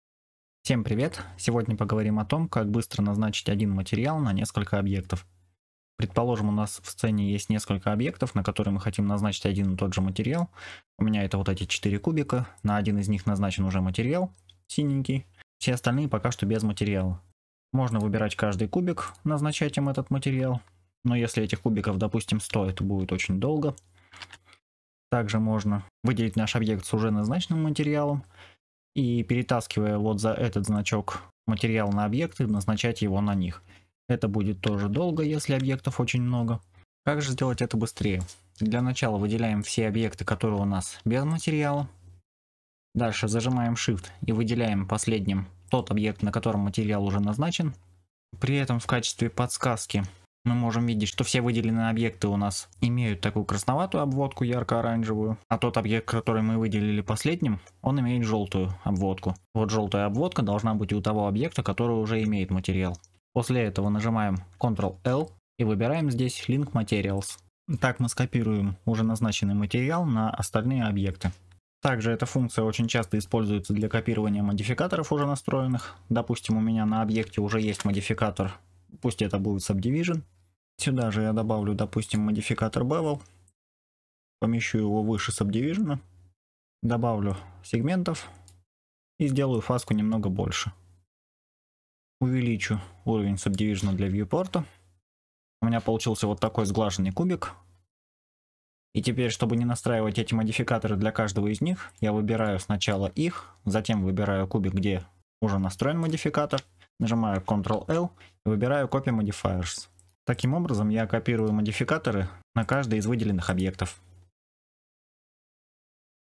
Назначить материал или модификатор сразу на несколько объектов Blender 3
00:00 Как назначить один материал сразу нескольким объектам
01:12 Как скопировать модификатор с настройками сразу нескольким объектам